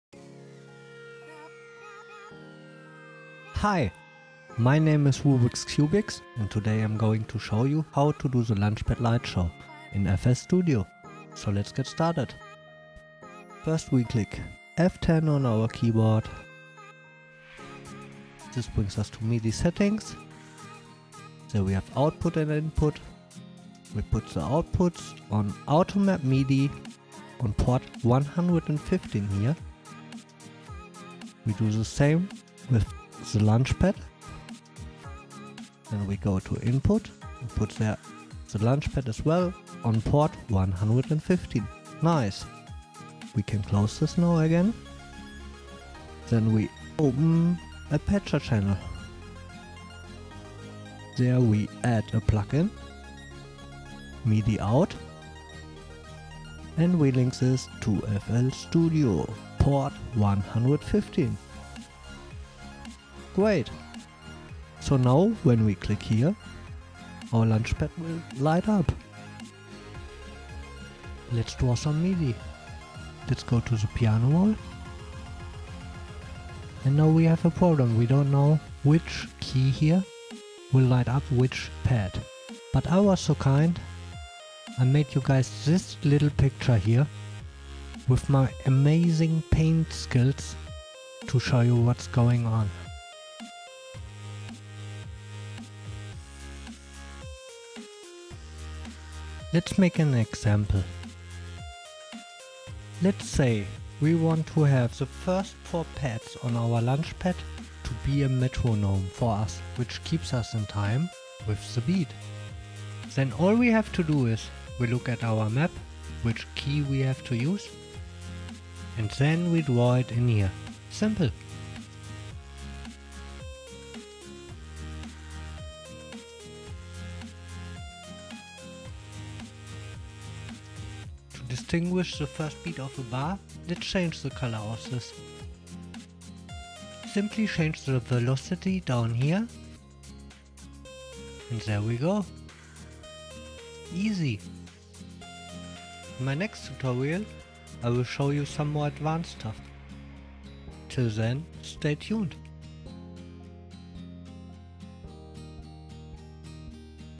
TUTORIAL: Launchpad Lightshow in FL Studio (Part 1: Basics)
In this first Tutorial I will show you the Basics of how to do a Launchpad Lightshow in FL Studio!